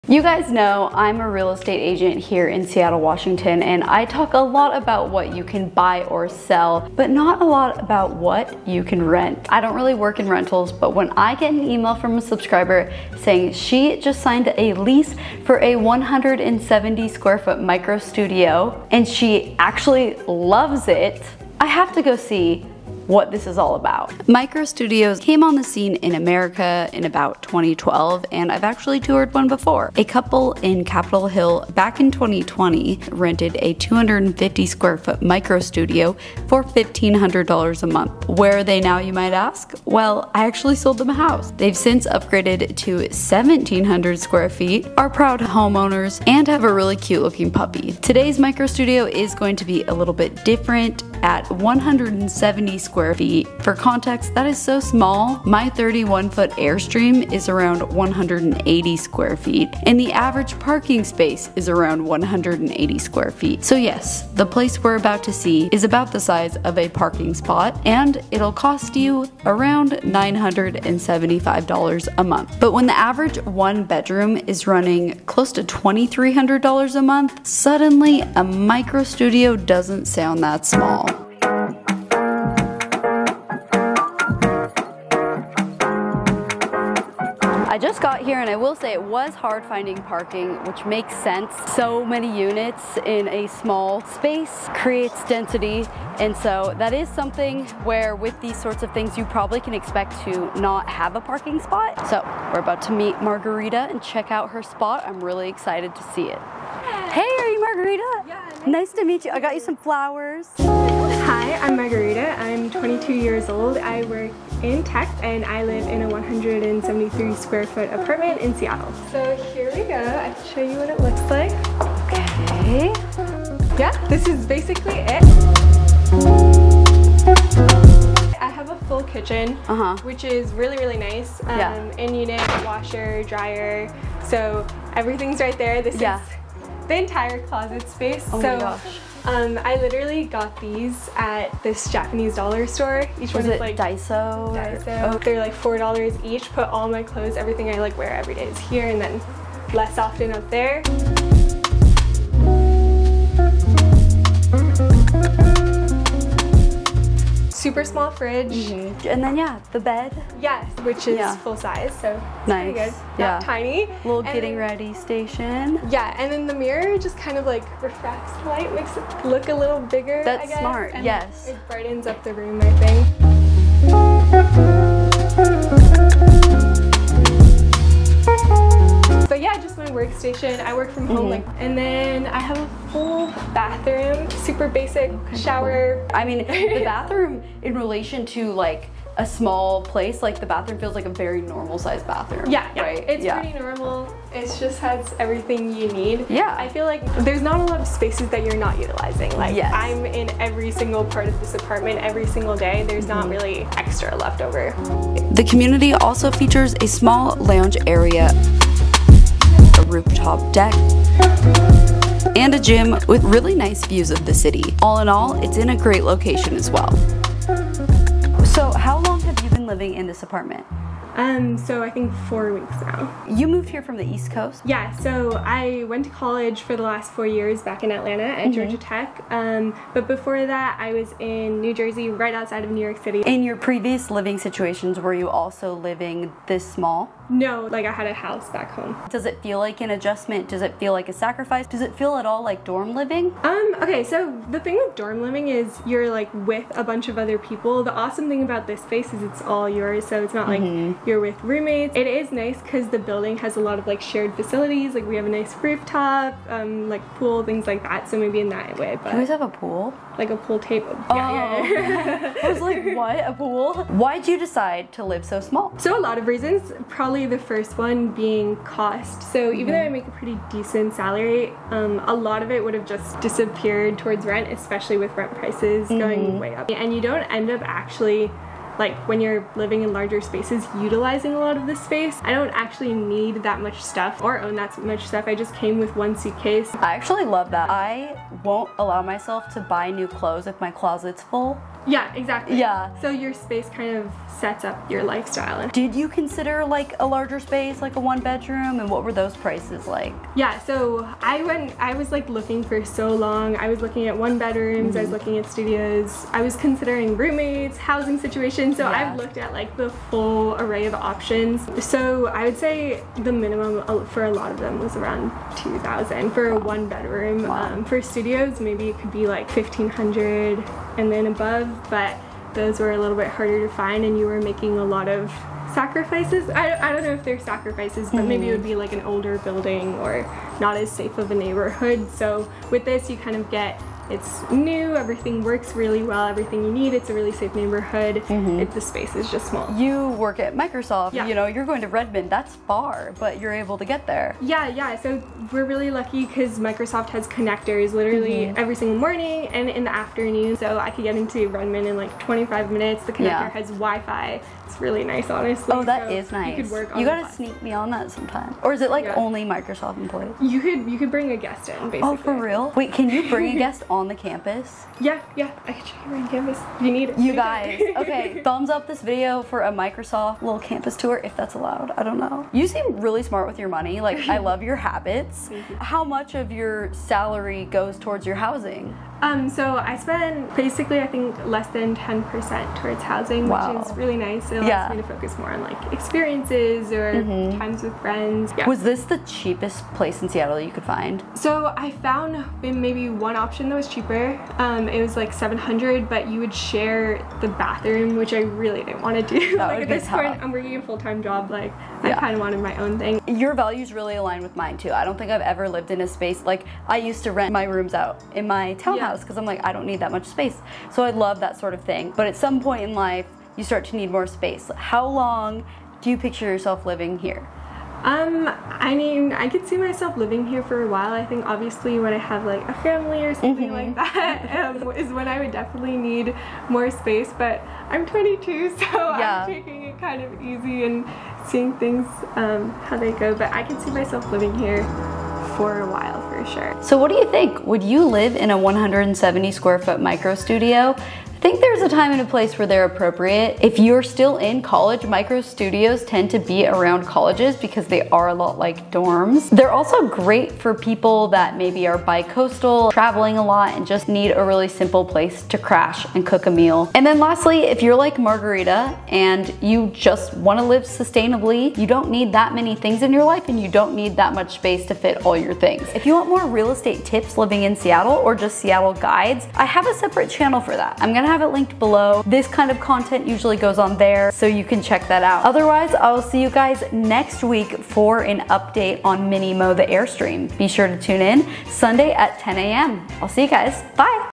Living In A 170 sqft Tiny Micro Studio on a Seattle Tech Salary
Today we are checking out a 170 sqft micro studio that a Seattle transplant, Margarita, opts to live in despite making a comfortable tech salary!